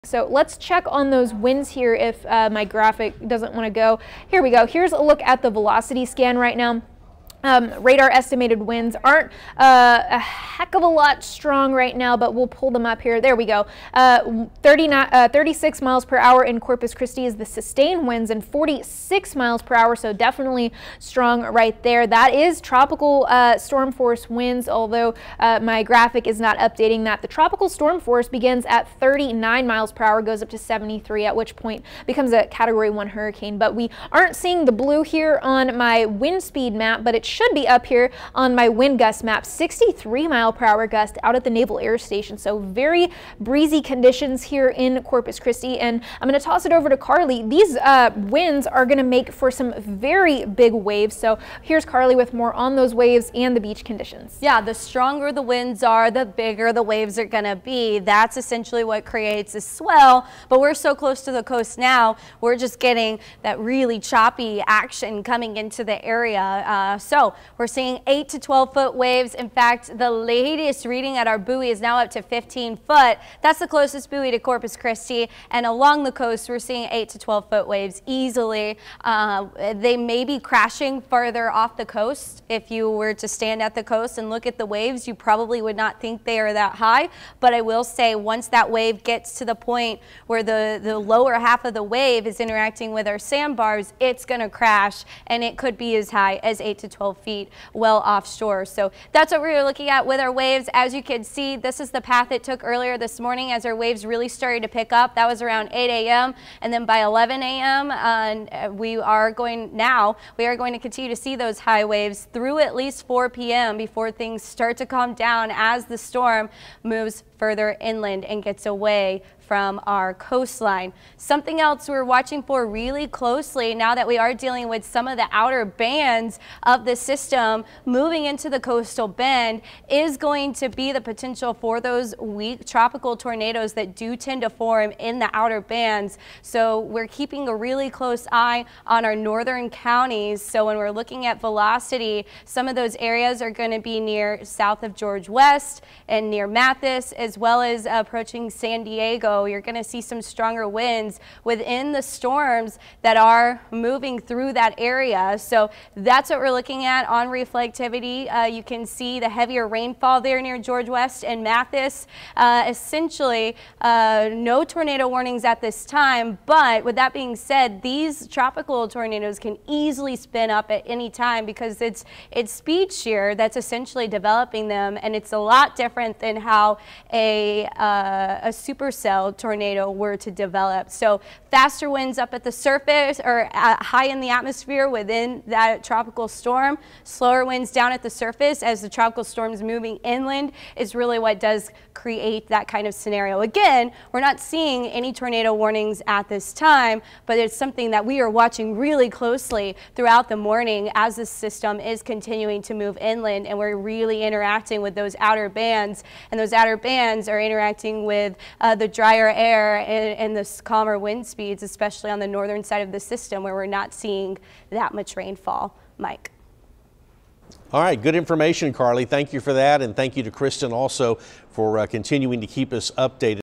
Strong Tropical Storm Harold winds make for strong waves, possible tornadoes
Meteorologists Kristin Walla and Carly Smith explain how strong Tropical Storm Harold winds make for strong waves and possible tornadoes.